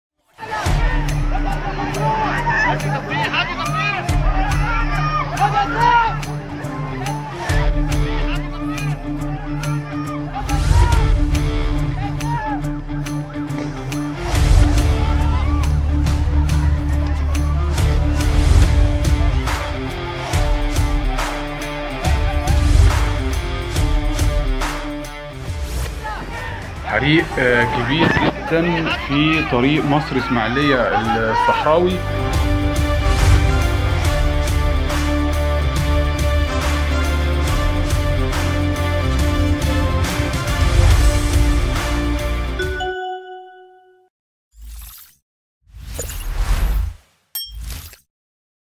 هذا الفيديو ليس لانفجار وقع في مدينة الحسكة شرق سورية
كابشن يوتيوب:
تتداول حسابات وصفحات على مواقع التواصل الاجتماعي، يوم 15 يوليو/تموز الجاري، مقطع فيديو تدّعي أنه لانفجار في مستودع أسلحة بمدينة الحسكة شمال شرقي سورية.

لكن الفيديو مضلل، إذ يعود إلى حادث حريق خط بترول على طريق القاهرة الإسماعيلية الصحراوى، بتاريخ 14 يوليو/تموز الجاري.